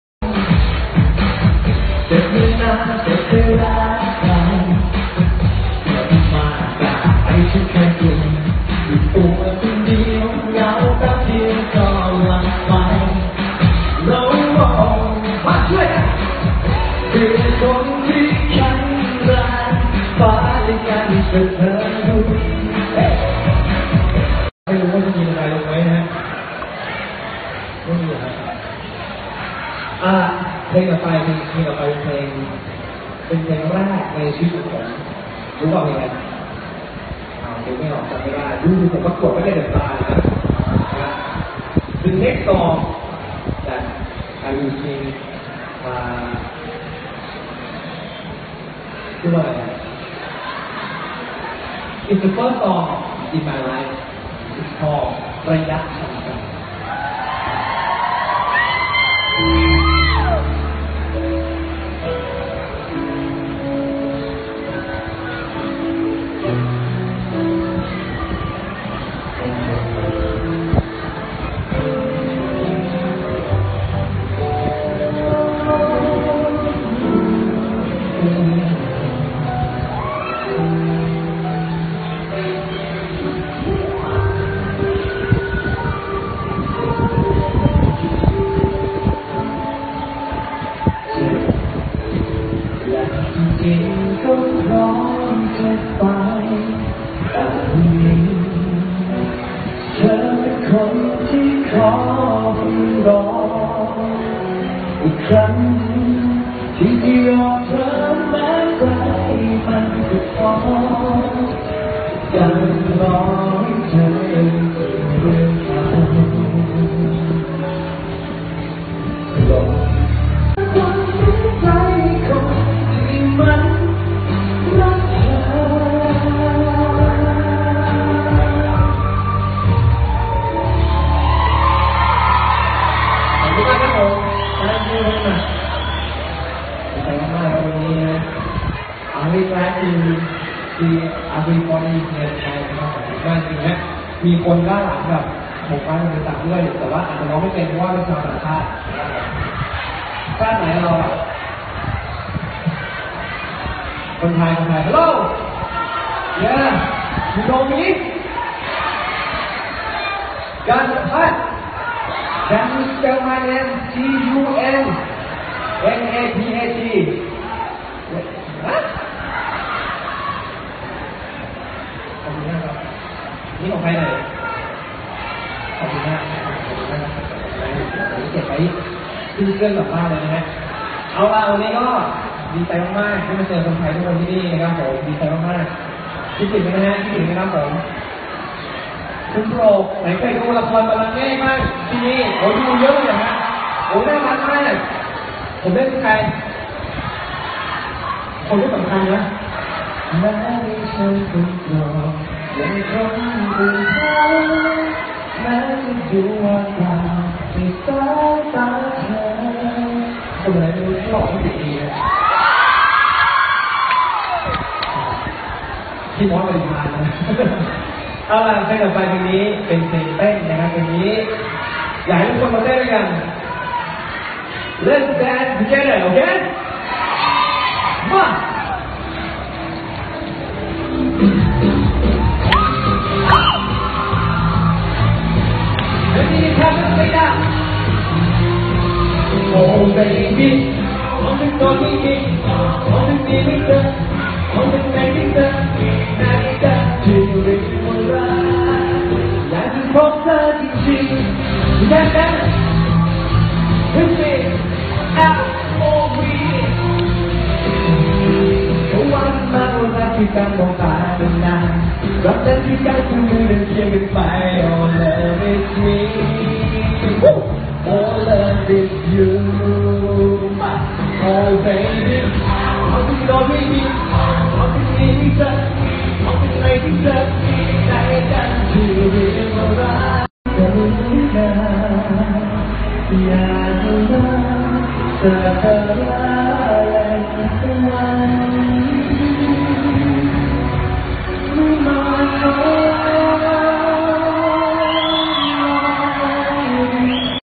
Gunnapat From dubai by Praew
รวมคลิป
เครดิต FB Praew Vanichpattumrattana ขอบคุณมากๆคะ 🙏🙏🙏
กัน นภัทร
วันพฤหัสที่ 3 มี.ค59
งาน : คอนเสิร์ต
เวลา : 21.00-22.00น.
สถานที่ : main stage global village dubai